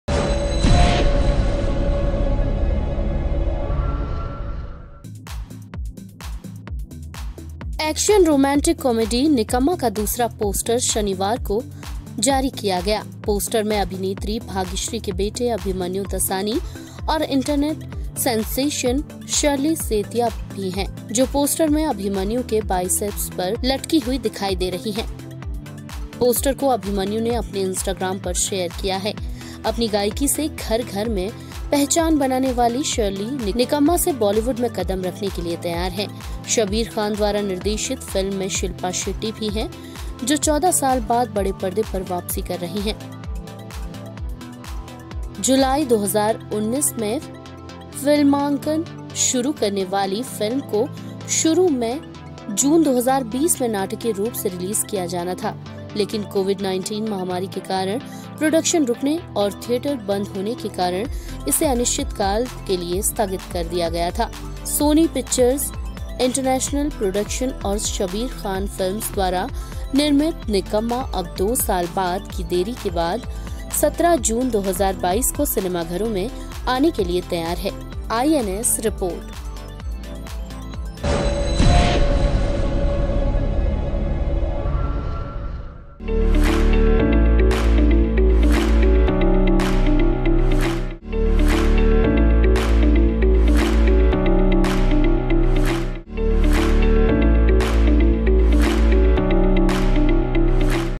Abhimanyu Dassani, Shirley Setia starrer 'Nikkamma' second poster out
The second poster of the action-romantic-comedy 'Nikamma' was released on Saturday.